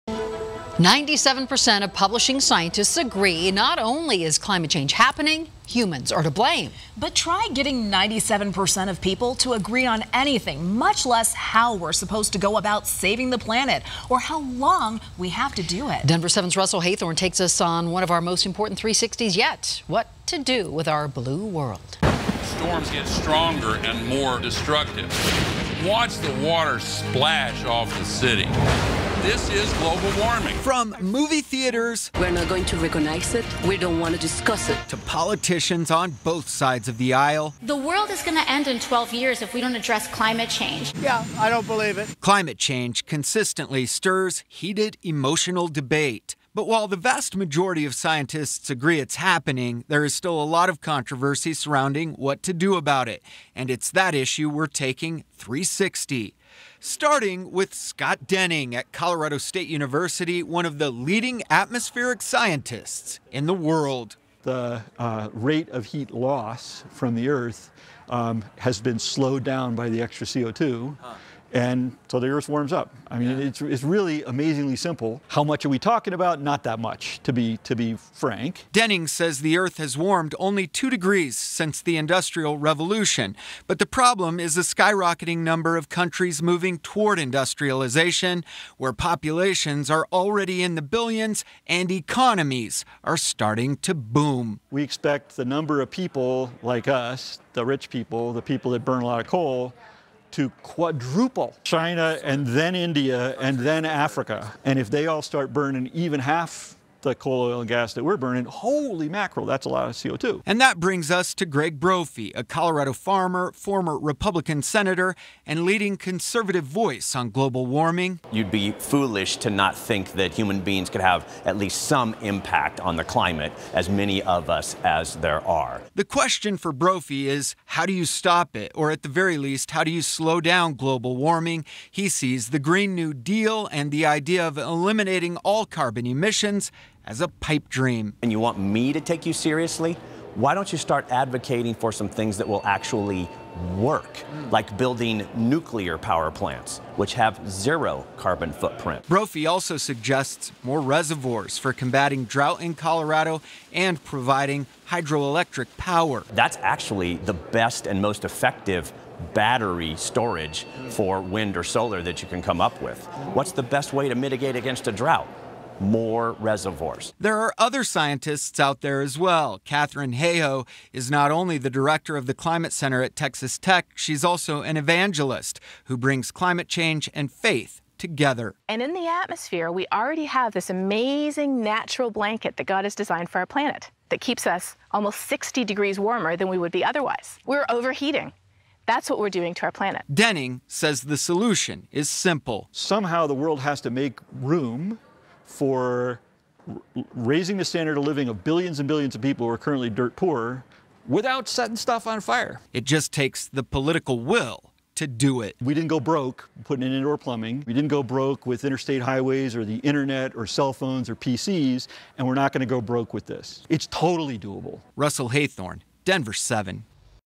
No magic wand for global warming as scientists and other experts debate solutions
Nearly 97 percent of publishing scientists agree: not only is climate change happening, but humans are also to blame.